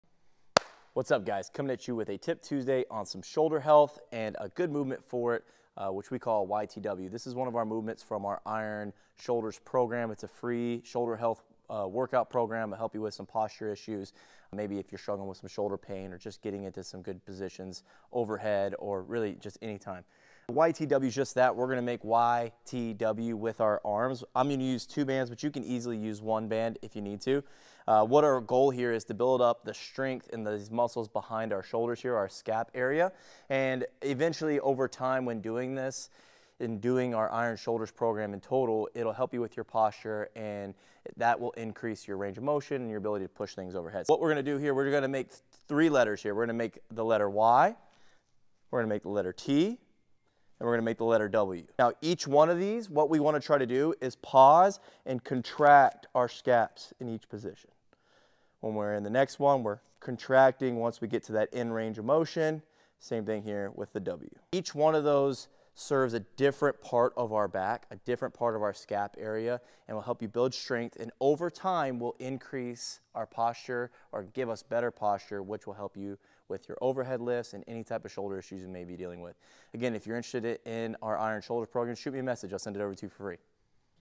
Technique Tuesday! 🎥
Today, we're focusing on shoulder health with a movement called YTW. This exercise is great for building shoulder strength and improving posture by targeting the scap area of your back. Want more tips on shoulder health? We have a free Iron Shoulders program that you can get by simply sending us a message. Let's build those strong, healthy shoulders together! 💪